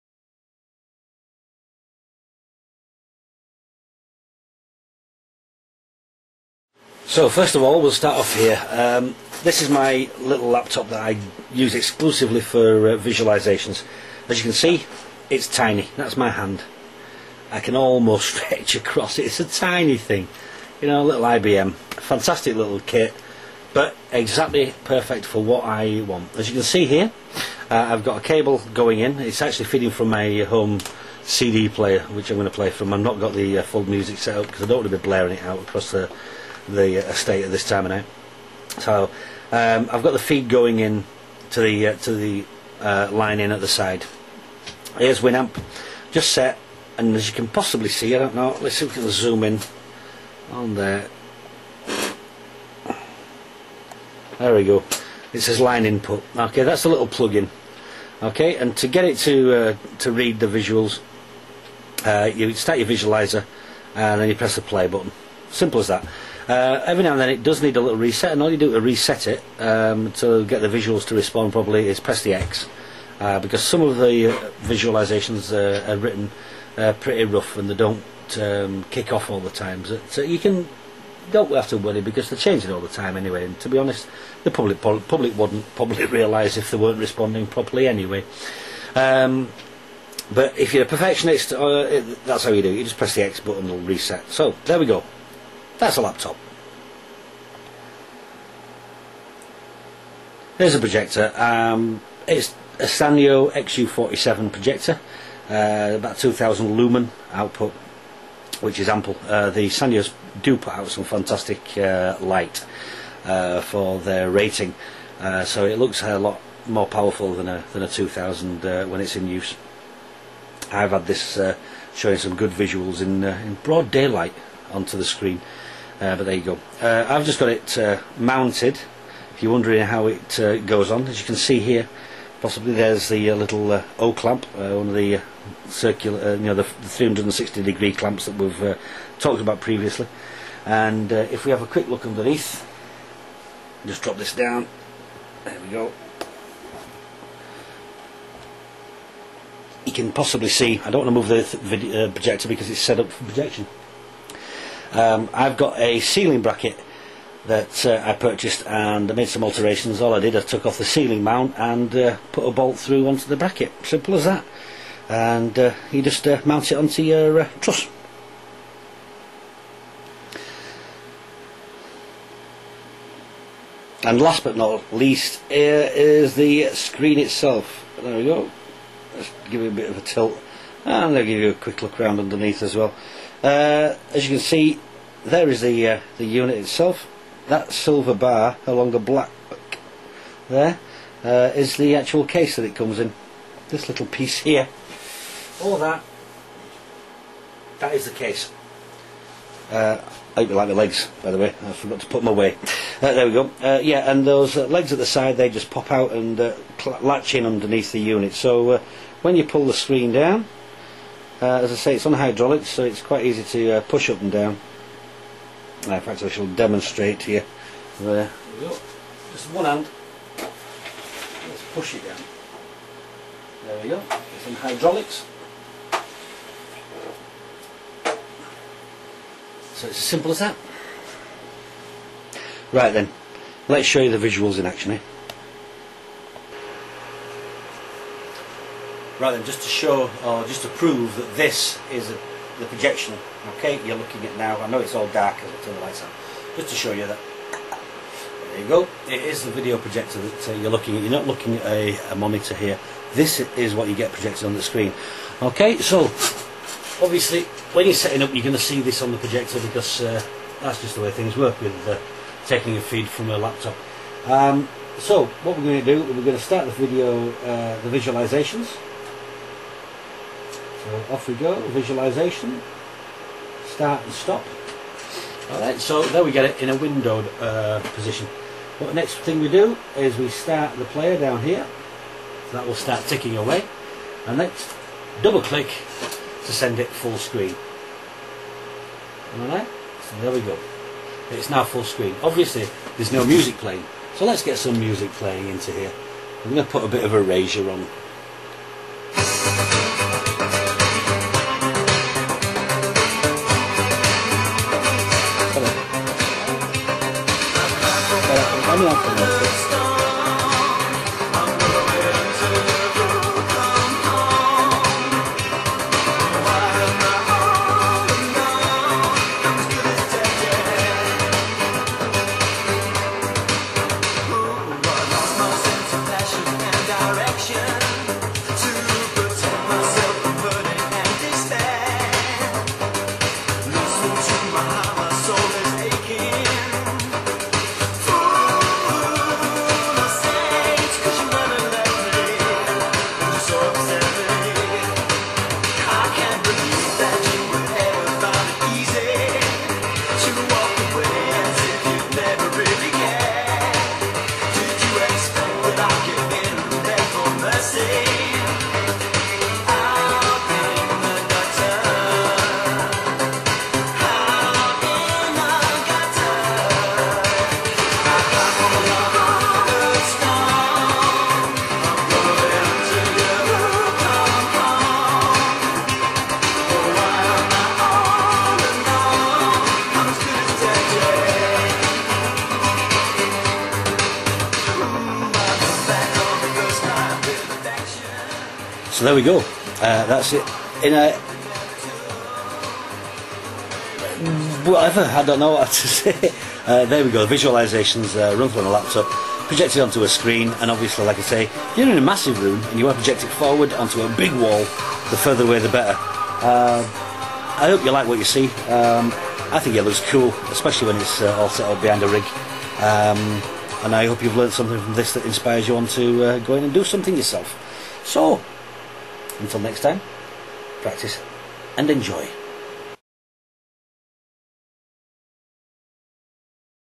Projector Visuals Pt2
Part 2 of the overview of my projection system. Part 1 is a discussion on the tech behind the system. Part 2 is a demo of the hardware. Please rate the video and leave you comments and questions.